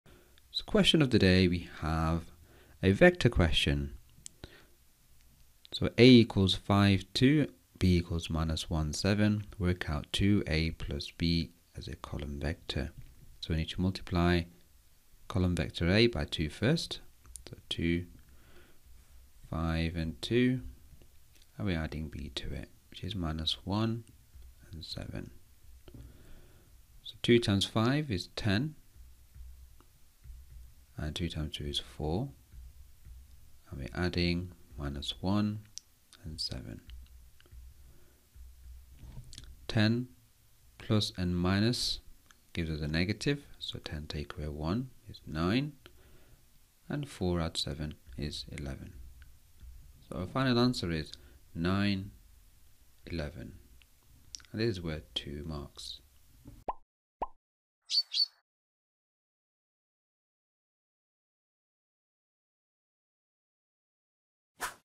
Column Vectors GCSE Maths
Column vectors are a very popular question in GCSE Maths. This column vector question is taken from GCSE Maths Edexcel May 2018 Foundation paper. In this question to work out the column vector we had to multiply the vector a by 2 them add vector b. Finally we had to write the answer as a column vector.